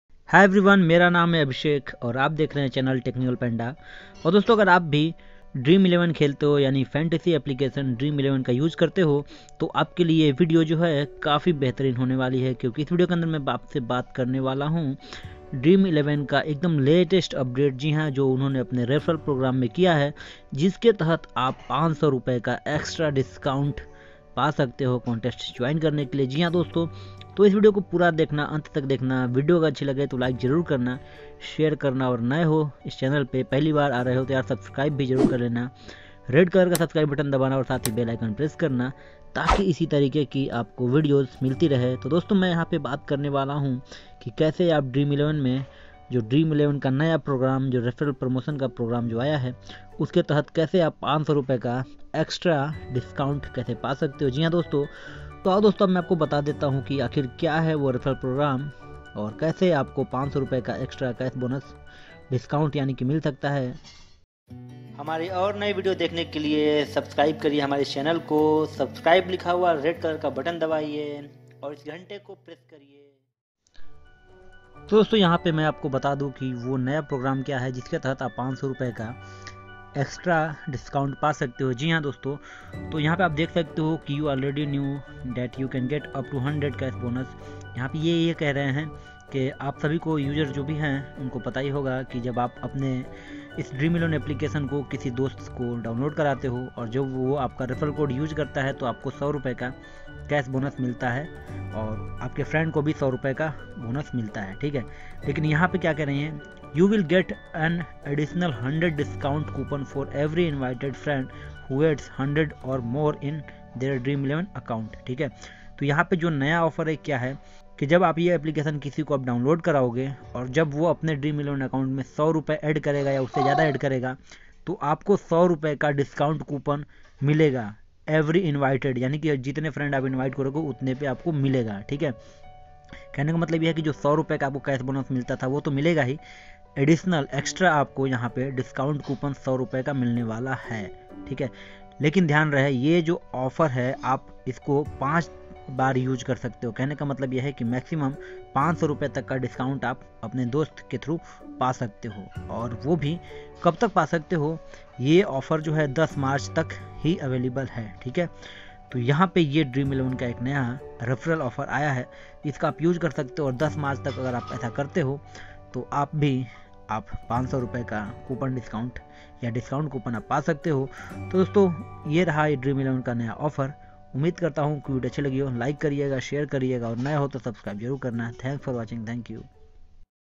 *UPDATE*- Dream11 New 2020 Update
Dream11 new Update New Features of dream11 dosto aaj ke is video me ham dream11 ke kuch update ke baare me baat karne wale hai.

TO DOSTO=

-Agar apko ye video achi lagi to like zaroor karna video ko
-Video ko apne friends or family ke saath share karna